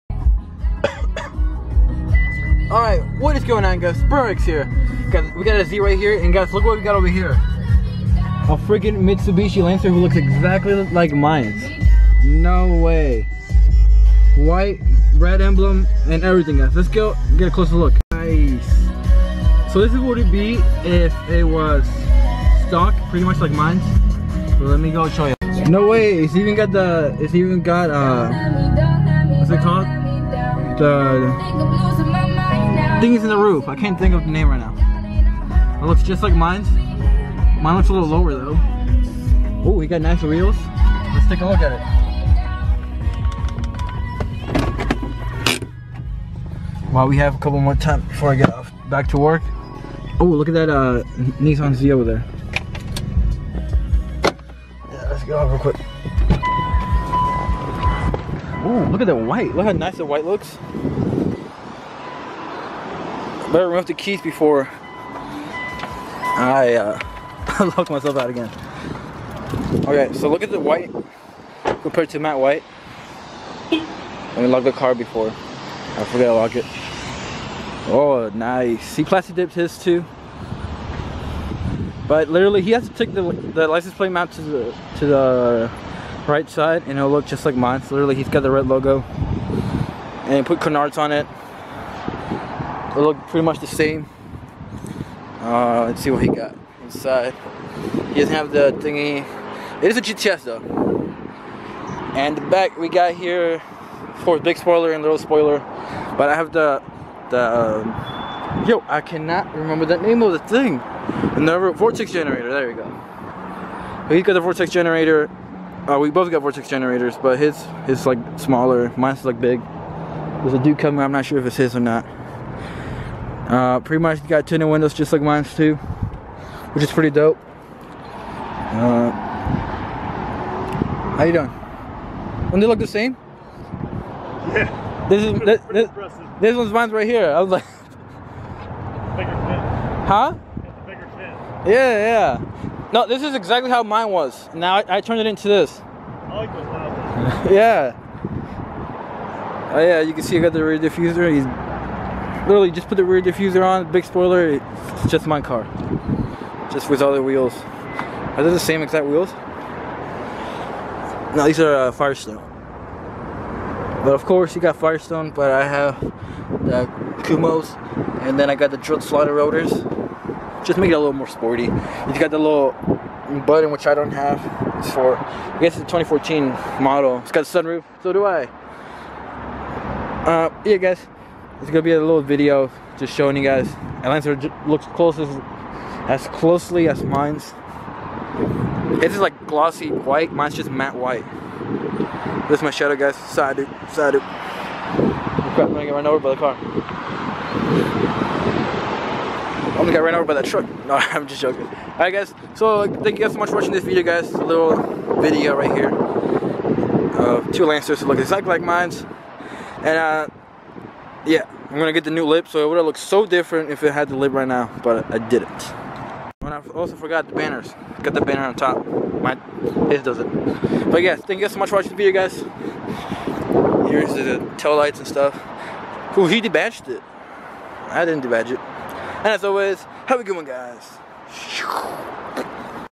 My twin!!! Can't tell the difference
Found a lancer that looks just like mine in the front, after working out saw a Nisan Z also

Pens stats W13 L6 OT3 Pts 29